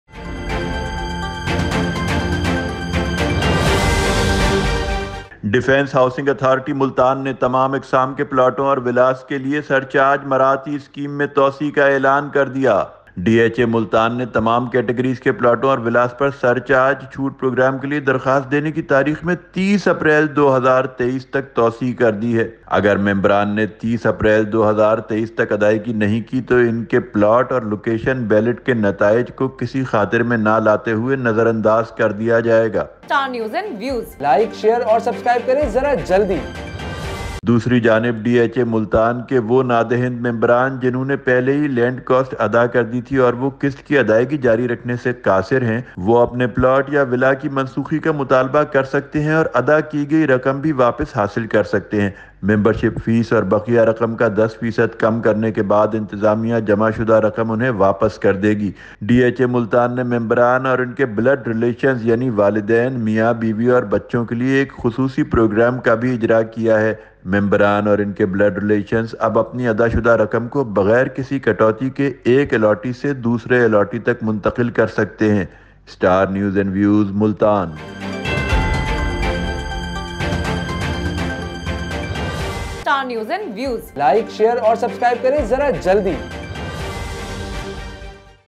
Extension Of Surcharge Incentive And Special Scheme For All Categories Of Plots And Villas
The government has announced an extension of surcharge incentives and special schemes for all categories of plots and villas. This move is aimed at promoting investment in the real estate sector and facilitating property buyers.